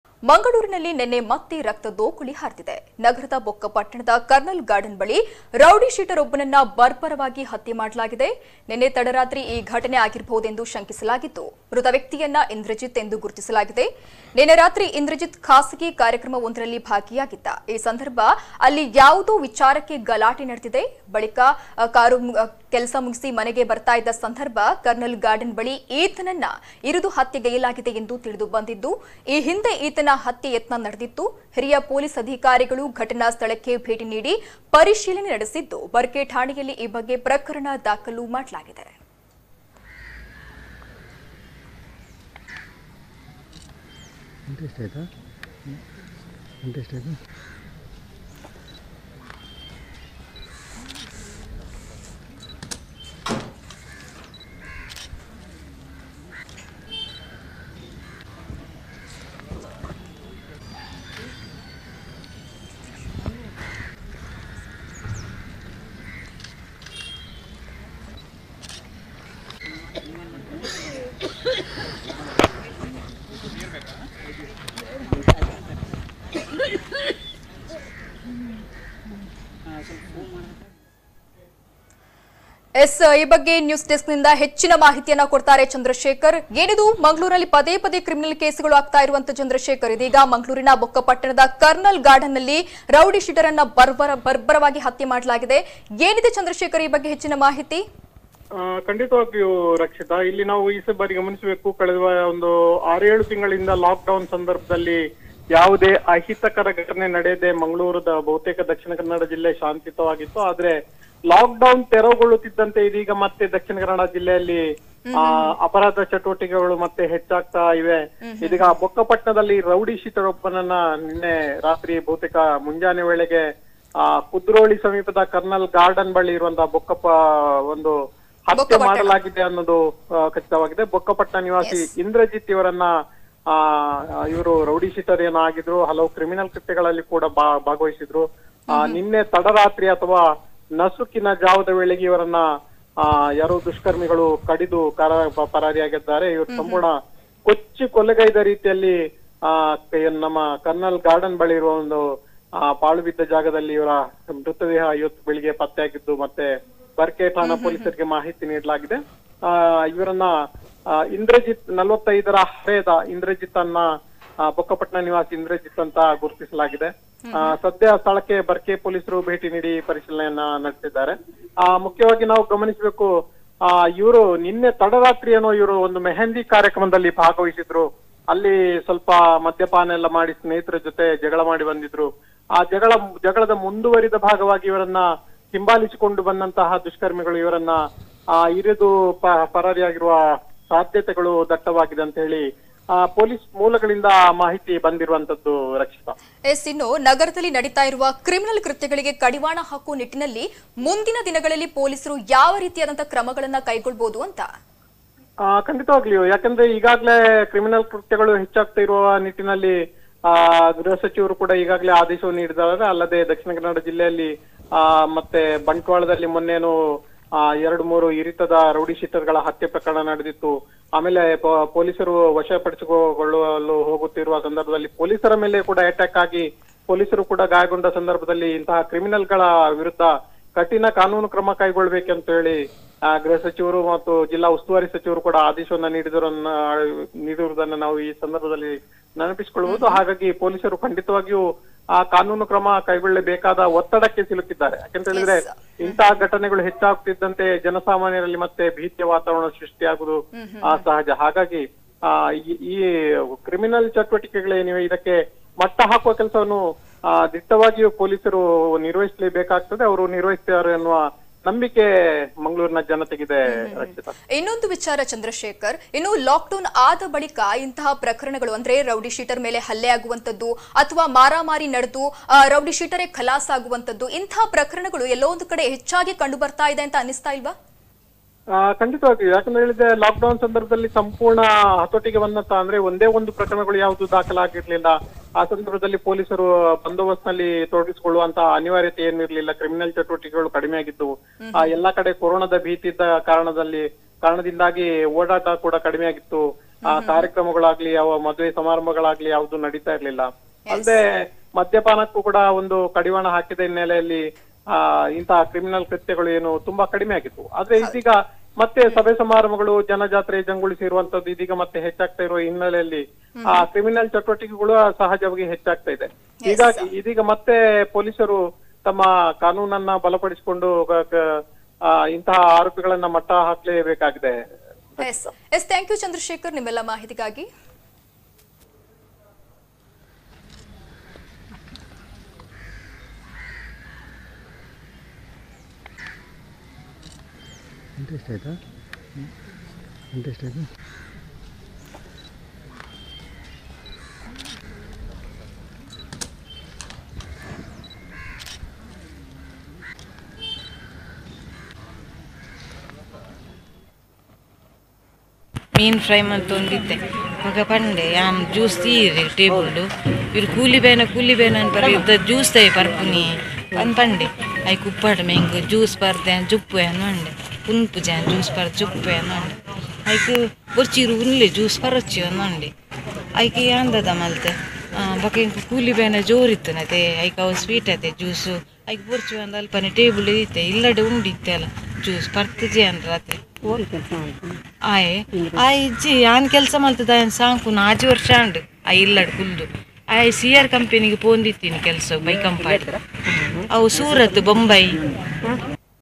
ಮಂಗಳೂರಿನಲ್ಲಿ ಮತ್ತೆ ಹರಿದ ನೆತ್ತರು....ರೌಡಿ ಶೀಟರ್ ನ ಭೀಕರ ಕೊಲೆ
ಮಂಗಳೂರಿನಲ್ಲಿ  ನಿನ್ನೆ ಮತ್ತೆ ರಕ್ತದೋಕುಳಿ ಹರಿದಿದೆ  ನಗರದ  ಬೊಕ್ಕಪಟ್ಟಣ್ಣದ ಕರ್ನಲ್ ಗಾರ್ಡನ್ ಬಳಿ ರೌಡಿಶೀಟರ್ ಒಬ್ಬನನ್ನು ಬರ್ಬರವಾಗಿ ಹತ್ಯೆ ಮಾಡಲಾಗಿದೆ. ನಿನ್ನೆ ತಡರಾತ್ರಿ ಈ ಘಟನೆ ಆಗಿರಬಹುದೆಂದು ಶಂಕಿಸಲಾಗಿದ್ದು ಮೃತ ವ್ಯಕ್ತಿಯನ್ನು ಇಂದ್ರಜೀತ್ (29) ಎಂದು  ಗುರುತ್ತಿಸಲಾಗಿದೆ.  ನಿನ್ನೆ ರಾತ್ರಿ ಇಂದ್ರಜೀತ್ ಖಾಸಾಗಿ  ಕಾರ್ಯಕ್ರಮವೊಂದರಲ್ಲಿ ಭಾಗಿಯಾಗಿದ್ದ. ಈ ಸಂದರ್ಭ ಅಲ್ಲಿ ಯಾವುದೋ ವಿಚಾರಕ್ಕೆ ಗಲಾಟೆ ನಡೆದಿದೆ. ಬಳಿಕ ಕಾಮ ಮುಗಿಸಿ ಮನೆಗೆ ತೆರಳುತ್ತಿದ್ದ ಸಂದರ್ಭ ಕರ್ನಲ್ ಗಾರ್ಡನ್ ಬಳಿ ಈತನನ್ನು ಇರಿದು ಹತ್ಯೆಗೈಯಲಾಗಿದೆ ಎಂದು ತಿಳಿದು ಬಂದಿದ್ದು, ಈ ಹಿಂದೆ ಈತನ ಹತ್ಯೆ ಯತ್ನ ನಡೆದಿತ್ತು . ಹಿರಿಯ ಪೆÇಲೀಸ್ ಅಧಿಕಾರಿಗಳು ಘಟನಾ ಸ್ಥಳಕ್ಕೆ ಭೇಟಿ ಣೀಡಿ ಪರಿಸೀಲನೆ ನಡೆಸಿದ್ದು ಬರ್ಕೆ ಠಾಣೆಯಲ್ಲಿ ಈ ಬಗ್ಗೆ ಪ್ರಕರಣ ದಾಖಲು ಮಾಡಲಾಗಿದೆ.